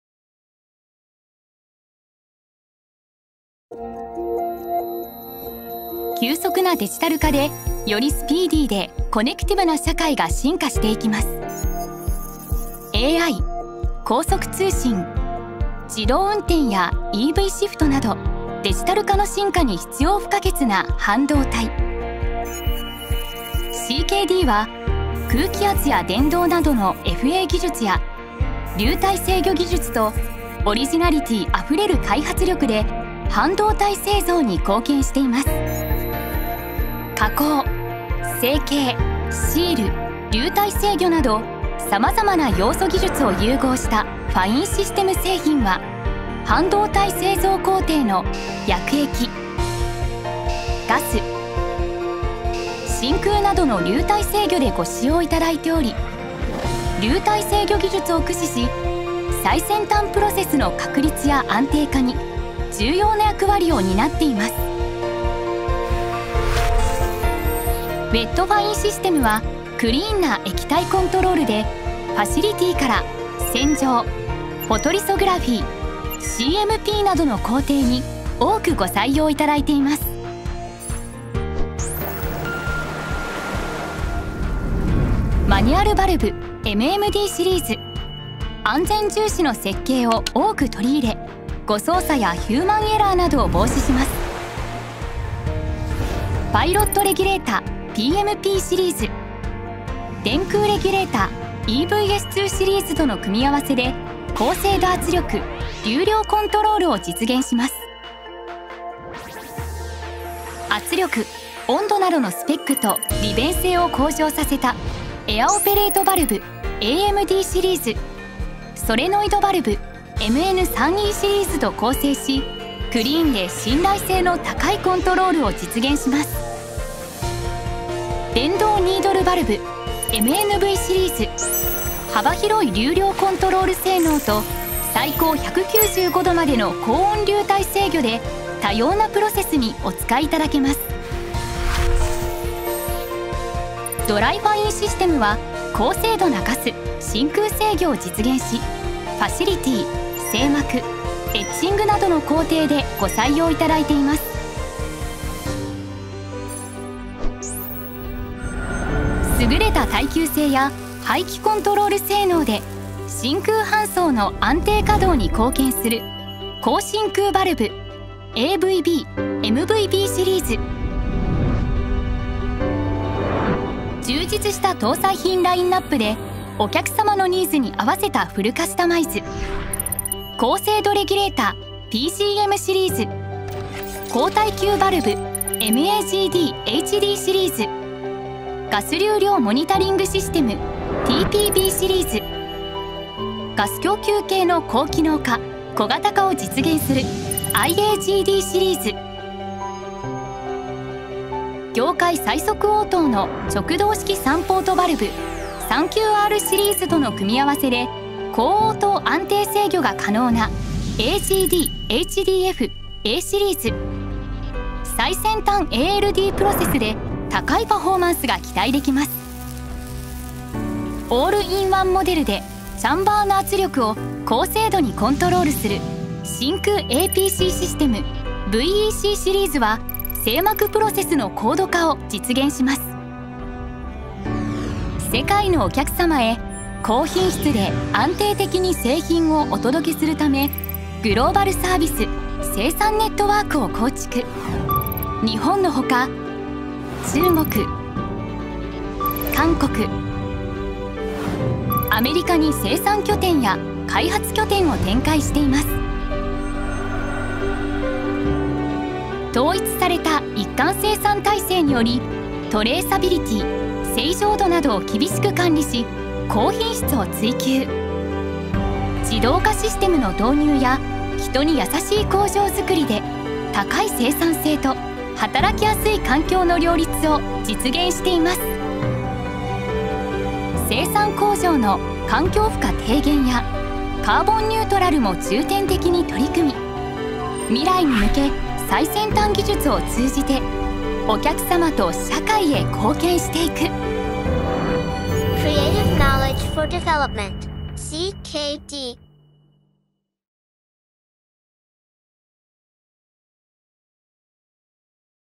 半導体製造工程向け商品のご紹介
半導体デバイス製造のファシリティから生産プロセス装置まで、薬液・ガス・真空・空圧制御トータルソリューションをご提案。